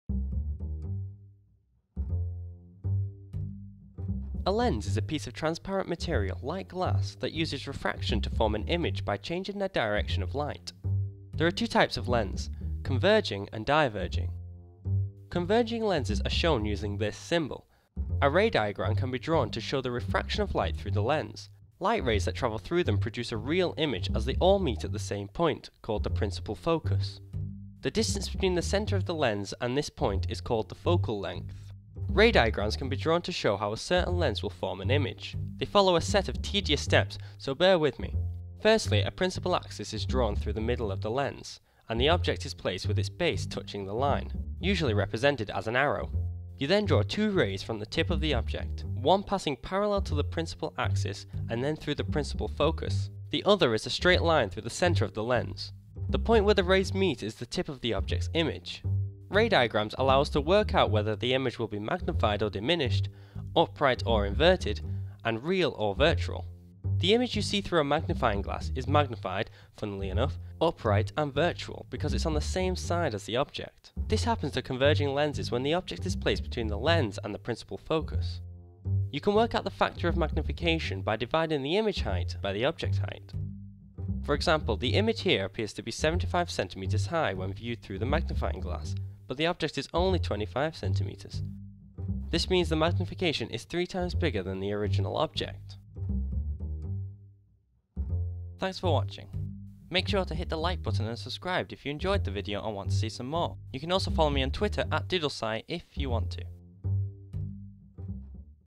Converging Lenses | GCSE Physics | Doodle Science
Doodle Science teaches you high school physics in a less boring way in almost no time!

GCSE Science

Script:
A lens is a piece of transparent material, like glass, that uses refraction to form an image by changing the direction of light.
There are two types of lens; converging and diverging.
Converging lenses are shown using this symbol. The lenses curve out on both sides. A ray diagram can be drawn to show the refraction of light through the lens. Light rays that travel through them produce a real image as they all meet at the same point, called the principle focus. The distance between the centre of the lens and this point is called the focal length.
Ray diagrams can be drawn to show how a certain lens will form an image. They follow a set of tedious steps so bear with me. Firstly, a principle axis is drawn through the middle of the lens and the object is placed with its base touching the line, usually represented as an arrow. You then draw two rays from the tip of the object. One passing parallel to the principle axis and then through the principle focus. The other is a straight line through the centre of the lens. The point where the rays meet is the tip of the object’s image. Ray diagrams allow us to work out whether the image will be magnified or diminished, upright or inverted and real or virtual.
The image you see through a magnifying glass is magnified (funnily enough), upright and virtual, because it’s on the same side as the object. This happens to converging lenses when the object is placed between the lens and the principle focus.
You can work out the factor of magnifications by dividing the image height by the object height. For example, the image here appears to be 75cm high when viewed through the magnifying glass but the object is only 25cm. This means the magnification is 3 times bigger than the original object.